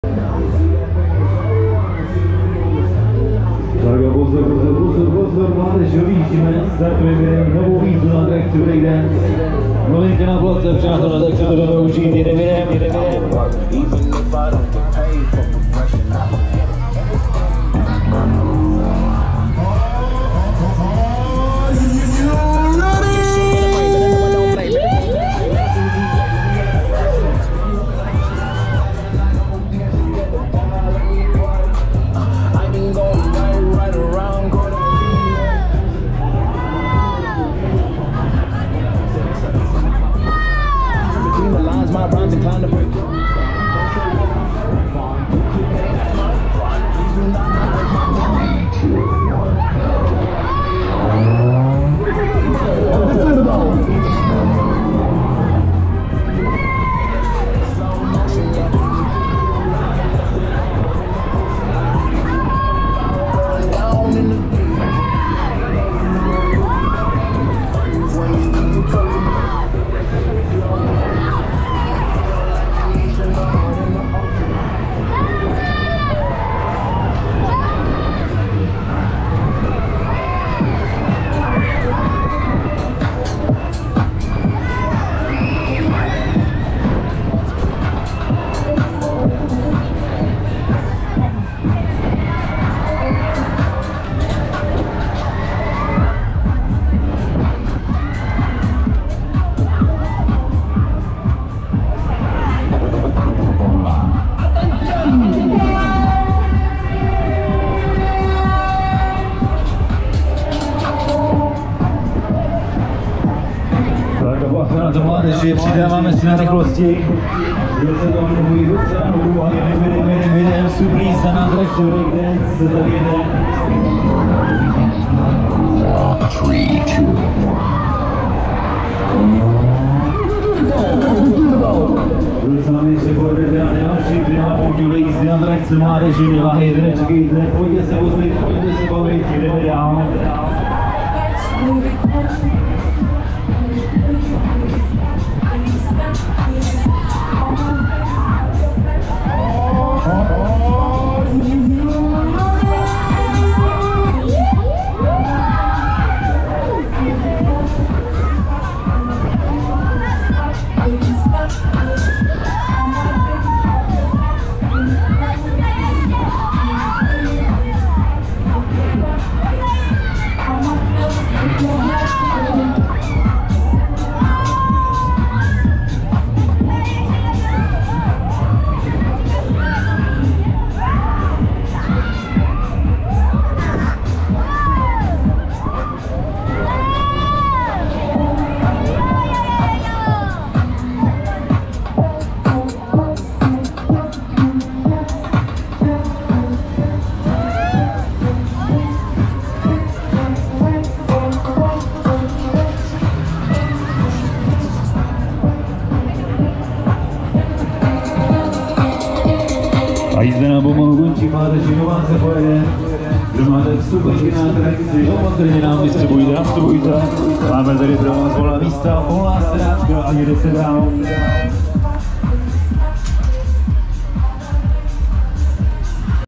Joker Dance No.1 - Lagron (on-ride) | Plzeňská pouť u sv. Jiří 2022
🎡 atrakce: Joker Dance (Kolmax - Break Dance)
😎 provozovatel/lunapark 🎠: L. Lagron
💶 jízdné: 100 Kč
📅 natočeno v sobotu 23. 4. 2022